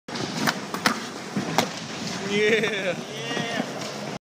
Halfcab Flip at Springfield, United States via ZonedIn World 1 Level 6 Aaron McGowen
is an app that documents your skateboarding journey into a real life video game. Here is Aaron McGowen doing Halfcab Flip at Springfield, United States to complete his Level 6 of World 1 of Zoned In App. and start your journey today.